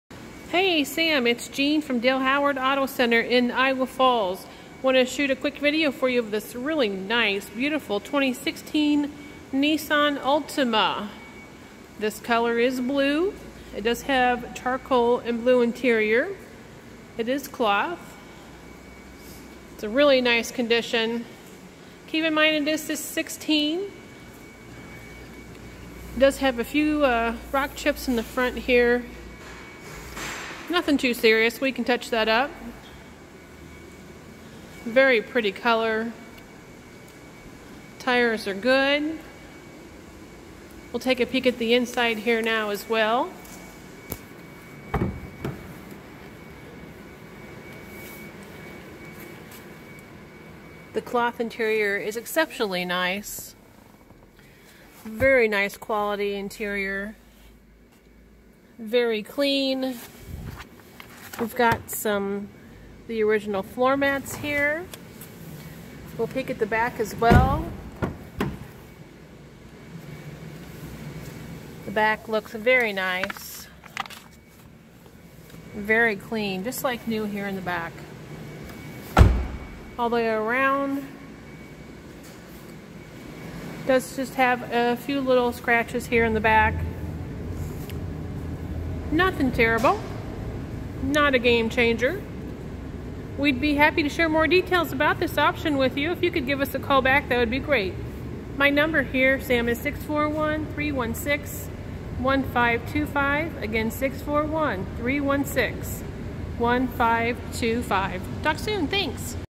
2016 Nissan Altima 2.5 S in blue at Dale Howard Auto Center in Iowa Falls
Video walk around